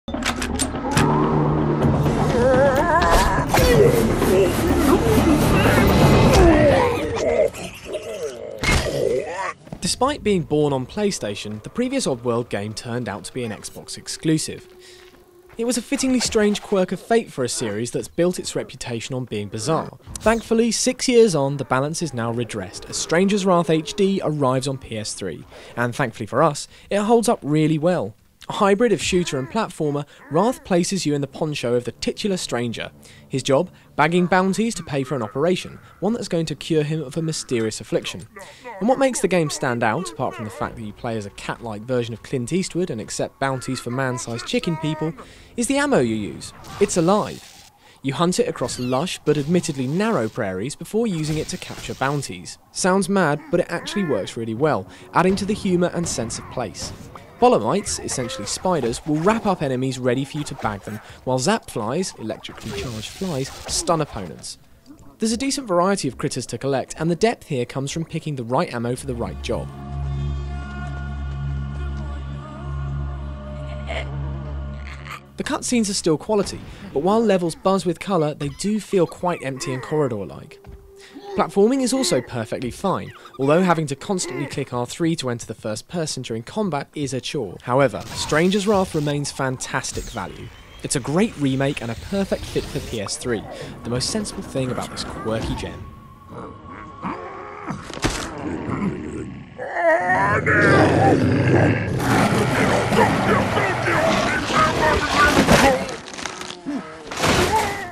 Oddworld: Stranger's Wrath HD review
Andy Hartup reviews the HD version of Oddworld Strangers Wrath HD on PS3. Voiced by David Boddington.

Did you catch Stranger's Wrath first time around? Excited to try some insect-projectile Wild West action?